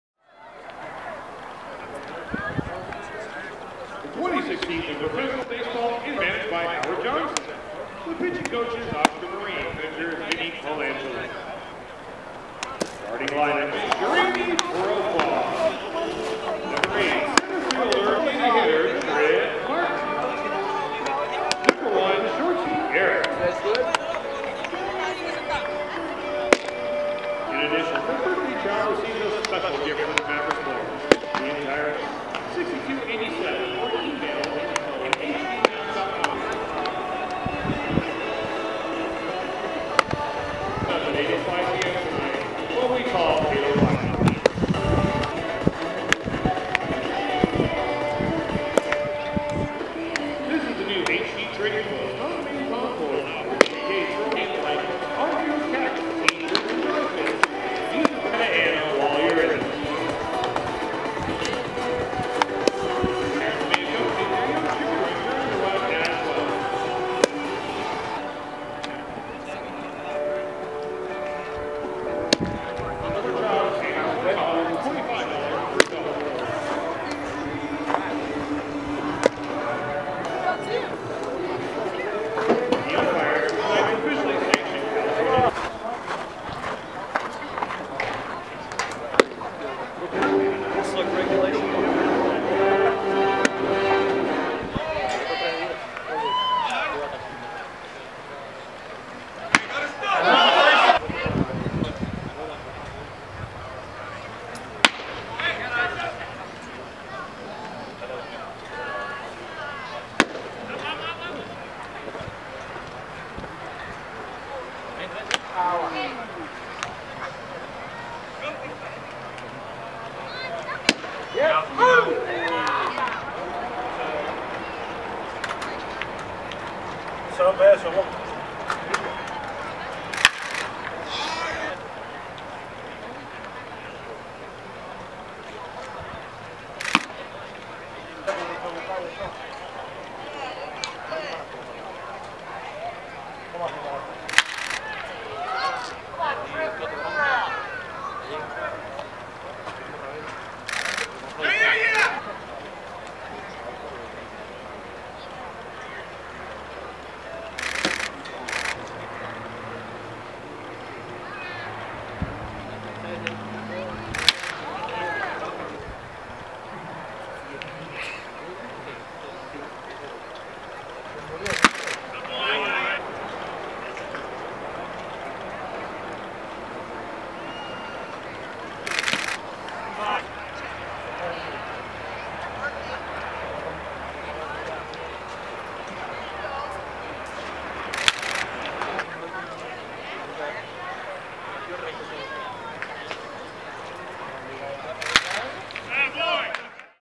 Matt Sergey - RHP - Oakland Athletics (Stockton Ports)
Oakland Athletics RHP pitching prospect Matt Sergey pitches for the High-A California League’s Stockton Ports in a recent game on the road against the High Desert Mavericks. Video by Bobby DeMuro.